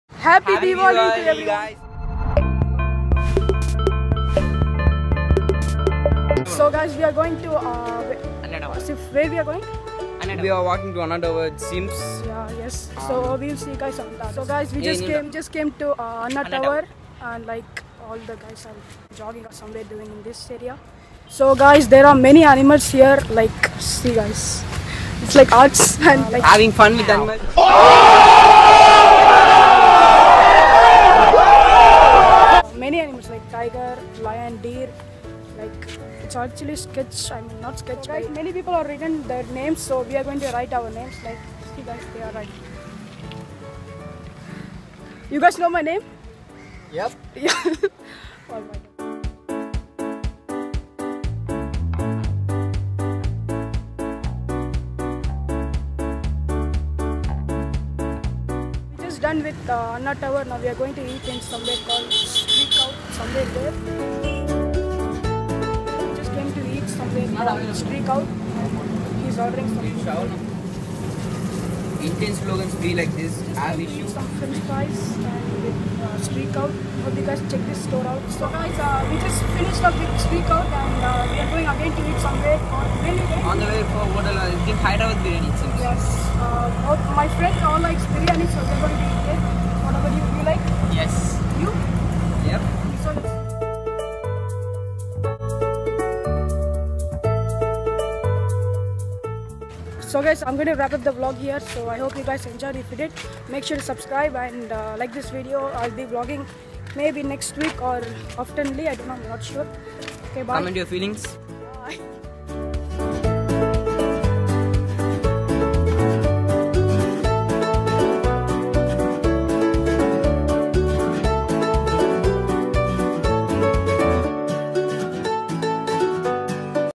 MY FIRST VLOG! ANNA TOWER CHENNAI
So guys it’s really short video! I seriously never had an experience vlogging in public with tons of people! It’s really awkward and I did it! Yeah! So guys please give your taughts and please do support me always!